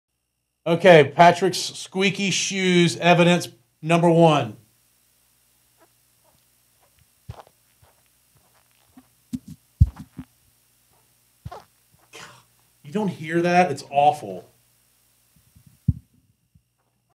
OMG!!! Listen to Patrick’s squeaky shoes
OMG!!!  Listen to Patrick’s squeaky shoes!  They’re driving us crazy 😳